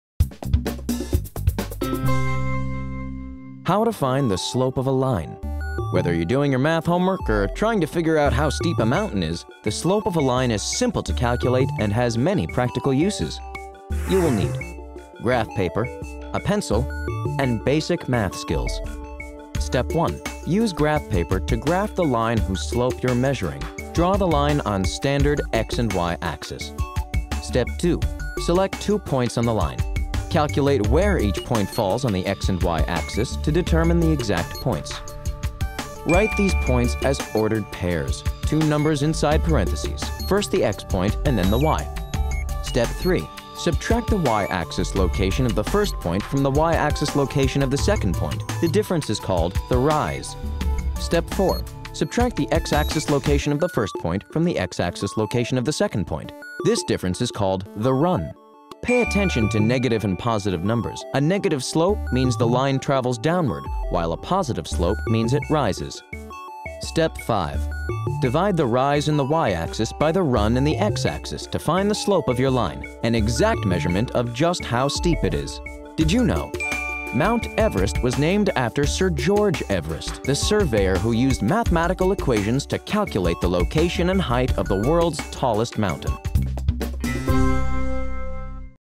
How to Find the Slope Of a Line
Whether you're doing your math homework or trying to figure out how steep a mountain is, the slope of a line is simple to calculate and has many practical uses.

Step 1: Graph the line whose slope you are measuring
Use graph paper to graph the line whose slope you are measuring. Draw the line on standard x-and y-axis.

Step 2: Select two points on the line
Select two points on the line. Calculate where each point falls on the x- and y-axis to determine the exact points.

Tip
Write these points as ordered pairs – two numbers inside parentheses. First the x point, and then the y.

Step 3: Subtract the y-axis locations of the two points
Subtract the y-axis location of the first point from the y-axis location of the second point. The difference is called the "rise."

Step 4: Subtract the x-axis locations of the two points
Subtract the x-axis location of the first point from the x-axis location of the second point. This difference is called the "run."

Tip
Pay attention to negative and positive numbers. A negative slope means the line travels downward, while a positive slope means it rises.

Step 5: Divide the rise by the run to find the slope
Divide the rise in the y-axis by the run in the x-axis to find the slope of your line, an exact measurement of just how steep it is.

Did you know? Mount Everest was named after Sir George Everest, the surveyor who used mathematical equations to calculate the location and height of the world's tallest mountain.